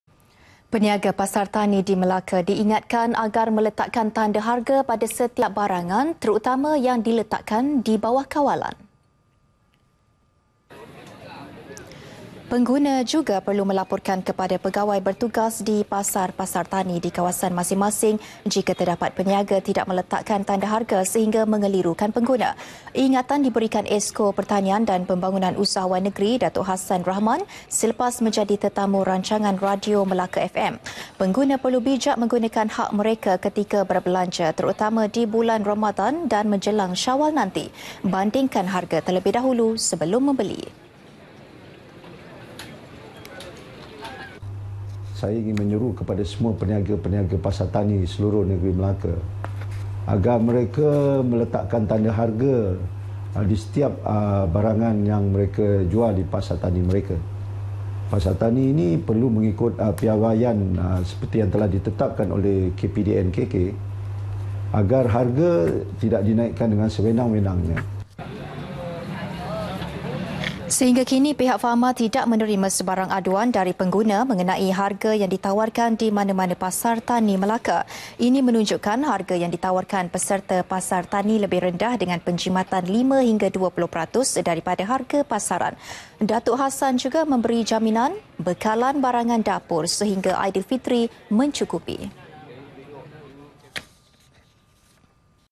PASAR TANI MELAKA- PENIAGA DIINGATKAN LETAK TANDA HARGA [1 JULAI 2016]
Peniaga Pasar Tani di Melaka diingat agar meletakkan tanda harga pada setiap barangan, terutama yang diletakkan dibawah kawalan.